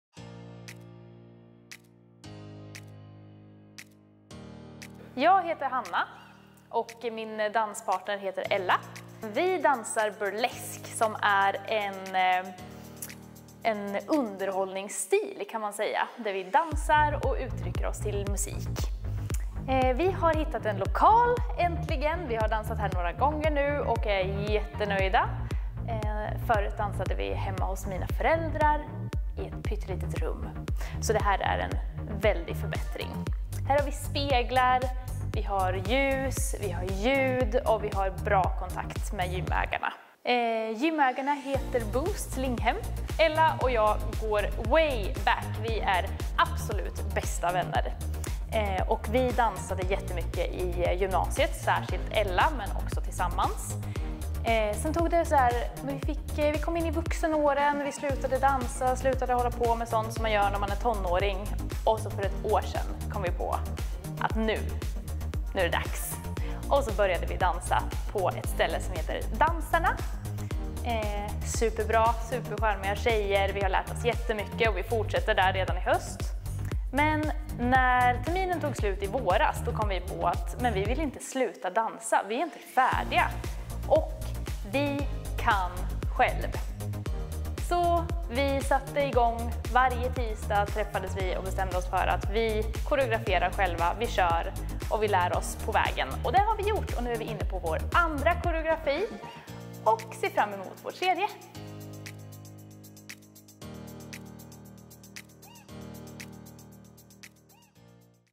Tissel Tassels presentation.
Hanna och Isabelle har startat en egen dansgrupp som heter Tissel Tassels där de koreograferar och utövar dansstilen Burlesque.
De har dansat tillsammans under många år men har precis börjat koreografera själva.
Här presenterar de sig själva och sin grupp.